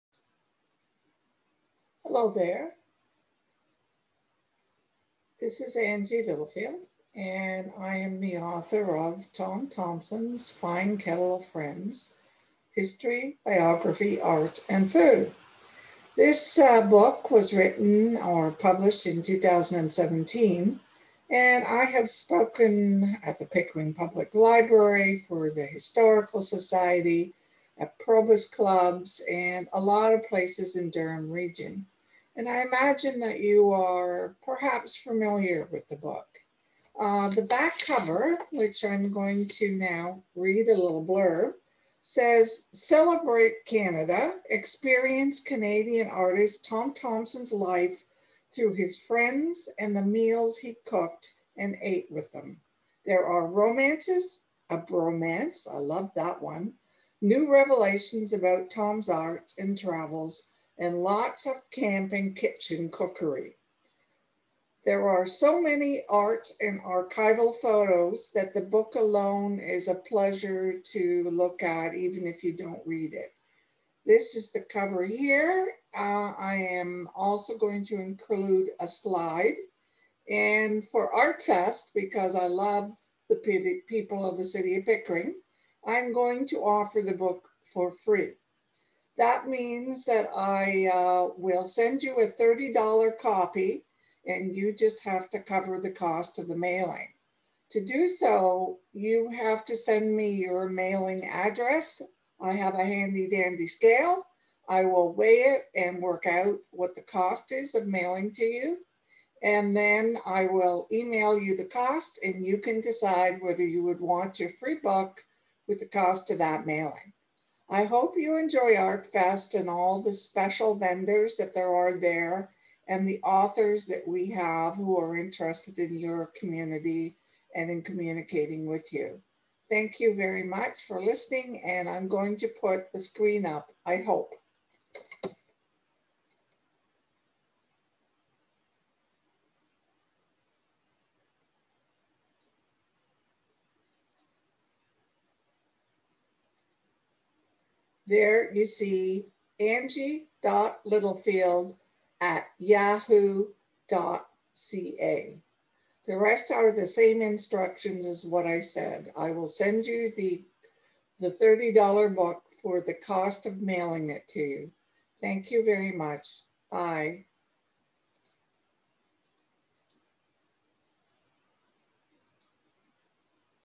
Angie Littlefield Artfest 2021
Angie talks about her latest book about Tom Thomson and Artfest 2021 in Pickering.